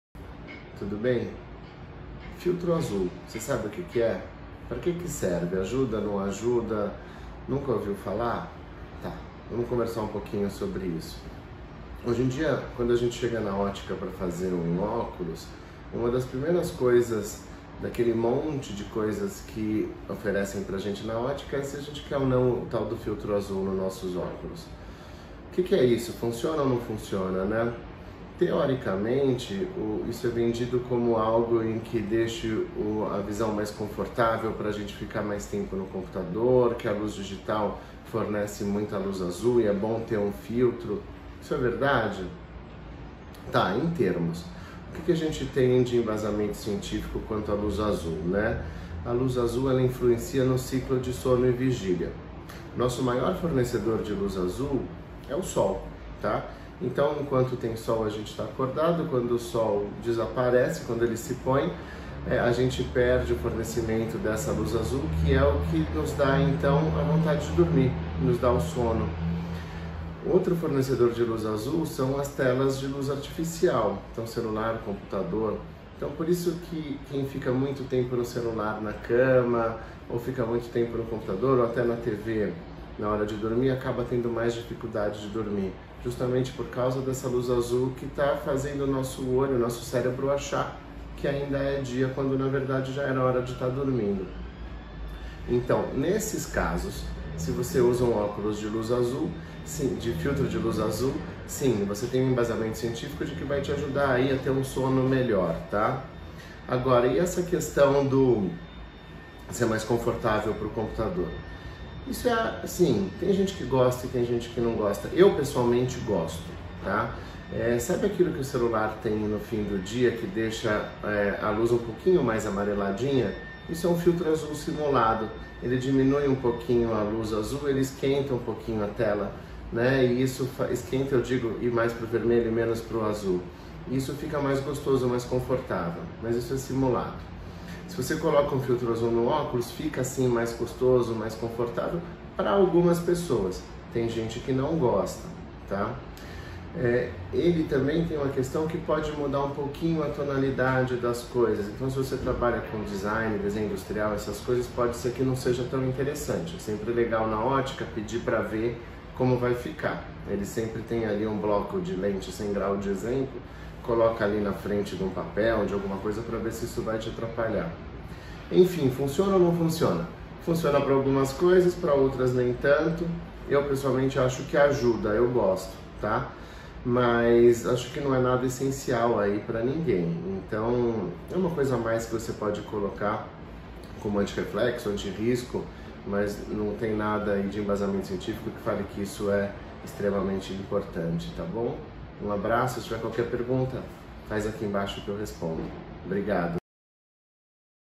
Filtro de Luz Azul nos Óculos?
Sim? Não?⁣
Vem assistir o vídeo de hoje e entender se essa é uma tecnologia essencial para quem usa óculos.⁣

Dr Hallim Féres Neto
CRM SP 117.127 / RQE 60732